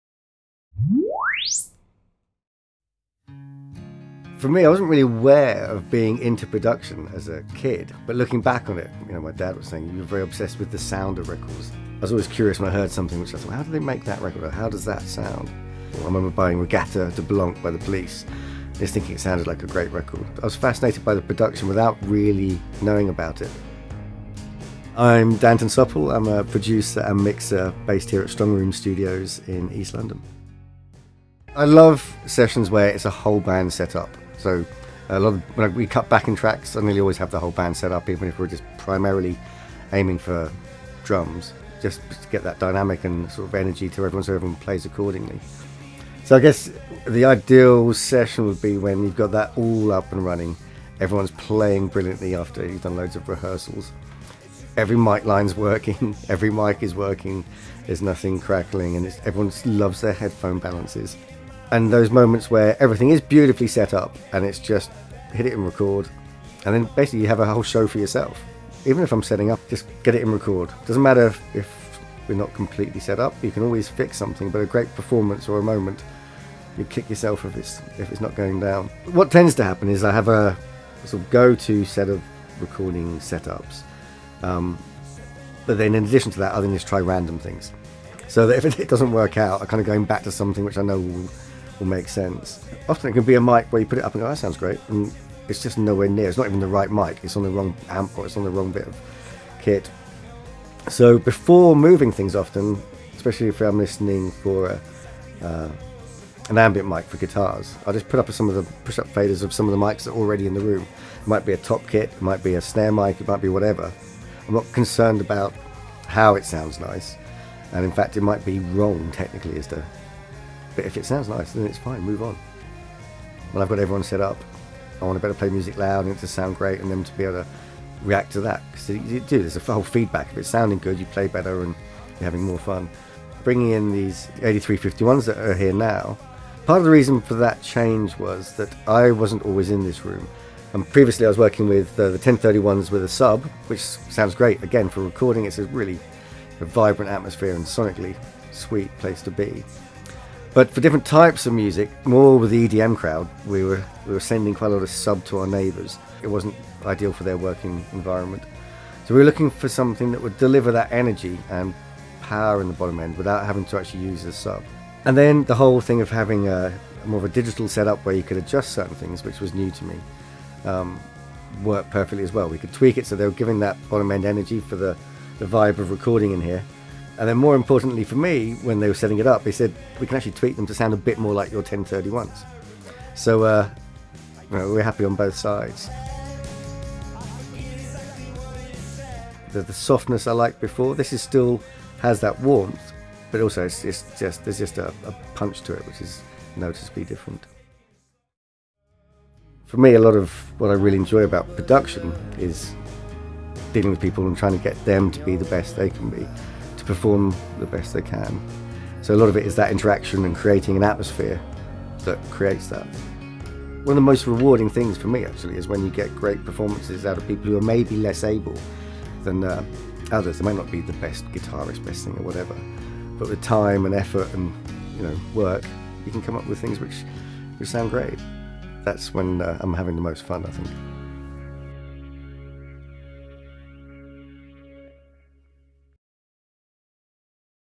One on One with Danton Supple | Genelec 8351| Interview
Danton Supple, the man behind albums by Coldplay, Ian Brown, Morrissey, Patti Smith, and U2, recently sat down One on One with Genelec to talk about his love for his 8351 SAM™ monitors, and how they help him produce his “very taut, very punchy sound”. Read more about the 8351 SAM™ monitors and The Ones product family